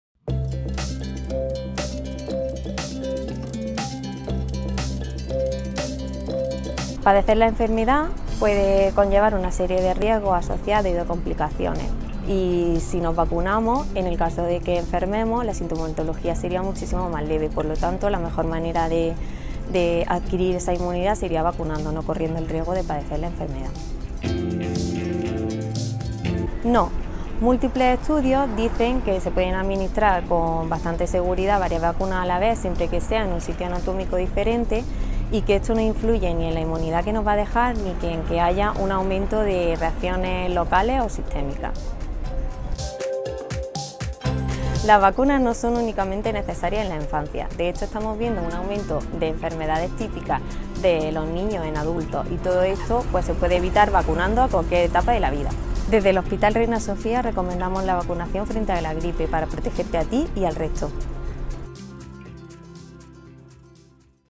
Falsos mitos sobre la vacunación de la gripe (2/2)
¡Nuevo vídeo! Continuamos respondiendo a preguntas y falsos mitos acerca de la vacunación contra la gripe.

Laura González, especialista en Medicina Preventiva del Hospital Universitario Reina Sofía de Córdoba responde en este vídeo algunas de las más frecuentes.

¡Compártelo!